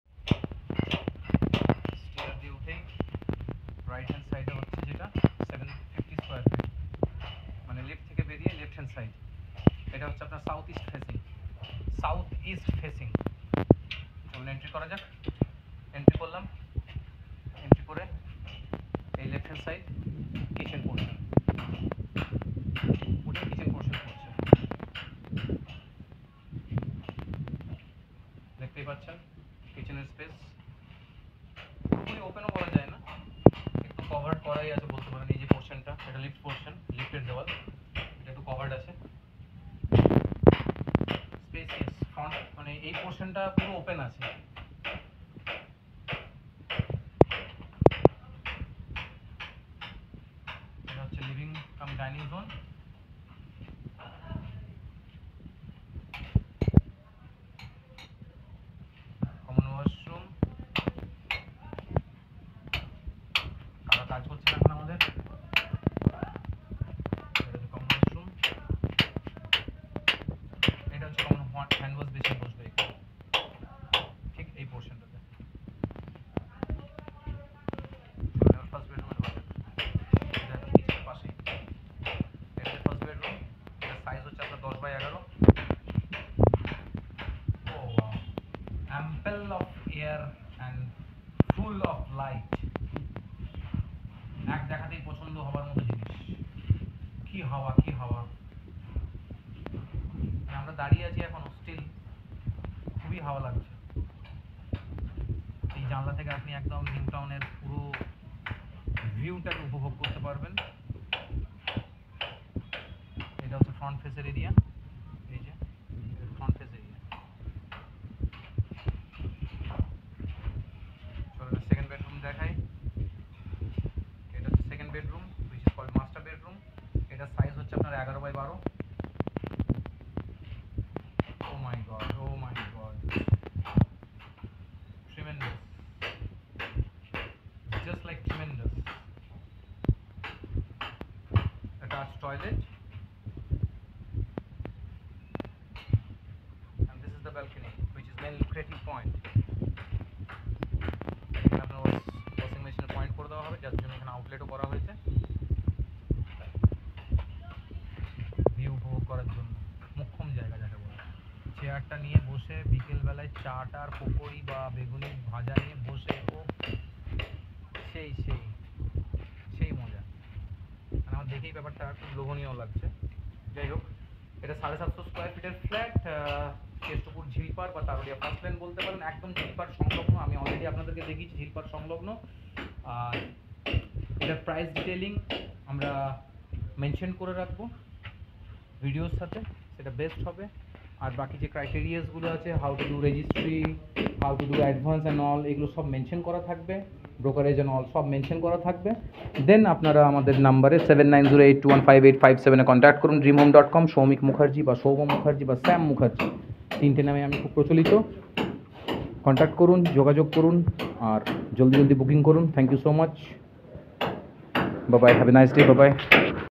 New Flat Sell....2bhk 2bath 1balcony750 sqft...price 26.50Lacs 🤙7908215857
2bhk 2bath 1balcony
750+ sqft
2nd,3rd  available
With lift
Bike parking space avlbl
Loanable

Price =
+- 26.25lacs(3500* delivered sqft)
2% brokerage applicable